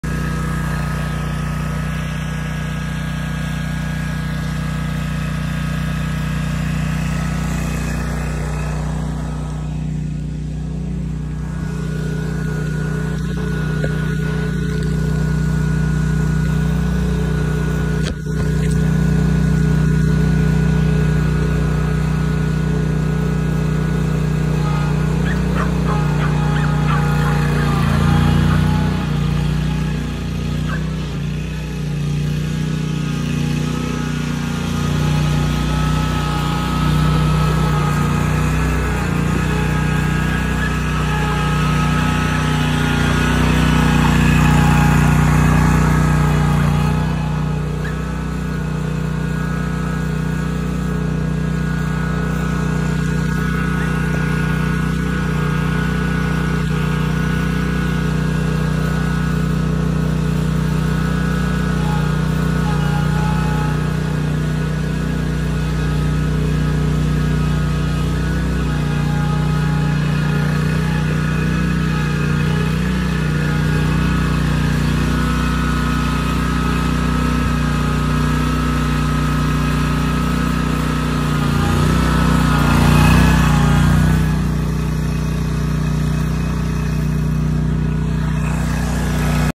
Wright 32 Inch Stand On Mower
Getting through 36 inch gates with a zero turn.